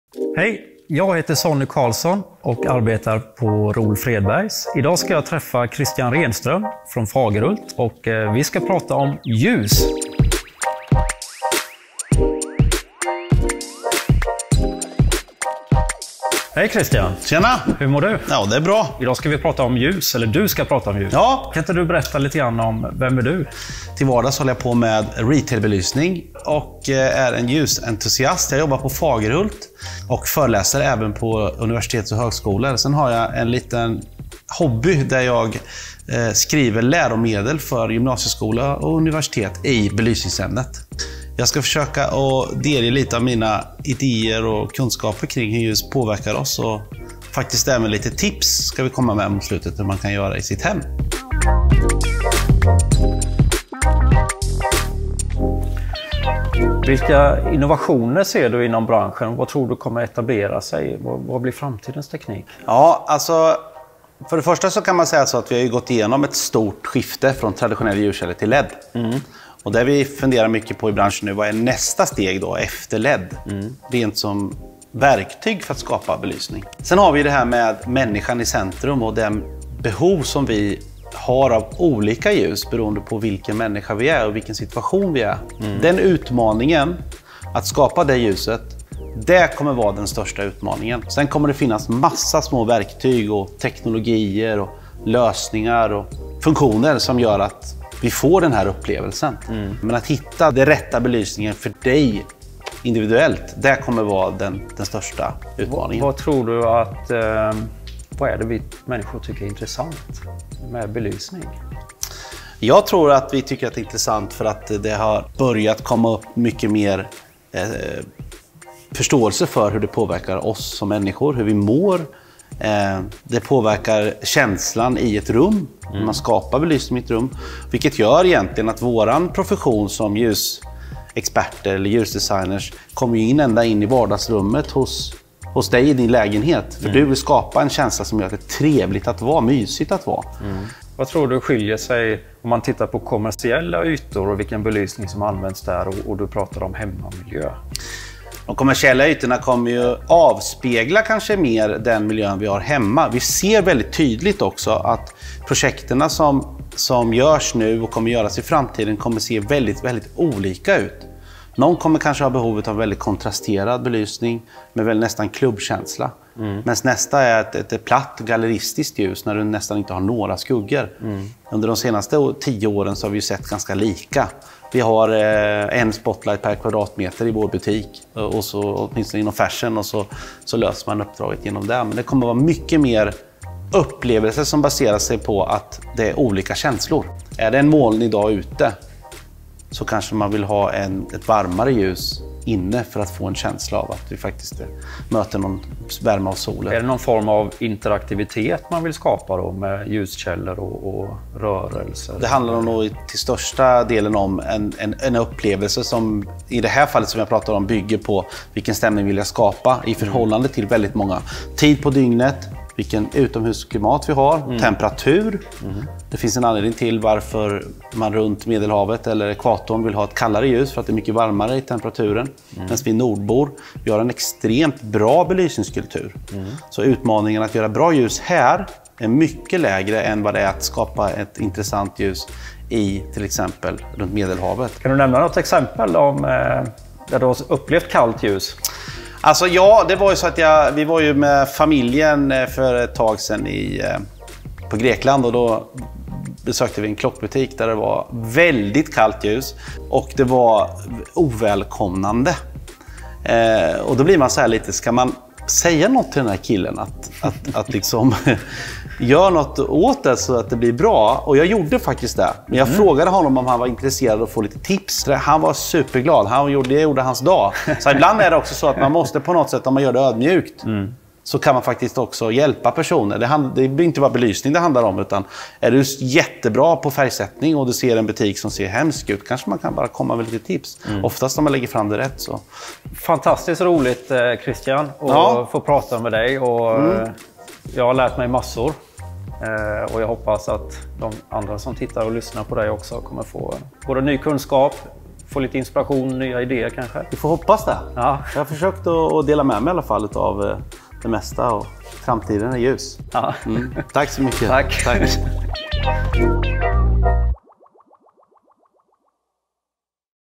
Lighting design | Kristian Renström from Fagerhult talks about technoligical trends in the industry
When we manage Turnkey deliveries of environments to retailers, restaurants, offices, hotels (etc), we interact with so many knowledgeable companies and people. We had an idea to interview experts in various fields to learn from them. This is what you will be able to see in a series of programs we call “ROL Trends”. We hope you will be as excited to listen and learn from them as we are.

The first interview is with Kristian Renström, a light design expert. Author of books about lighting, Lecturer at universities (KTH Royal Institute of Technology & Jönköping University), and working at Fagerhult for many years. Don´t miss out on Kristian sharing information with us.

In the coming weeks, we will share the Fagerhult project, divided into six different films.

Location:
Fagerhult Showroom, Jönköping, Sweden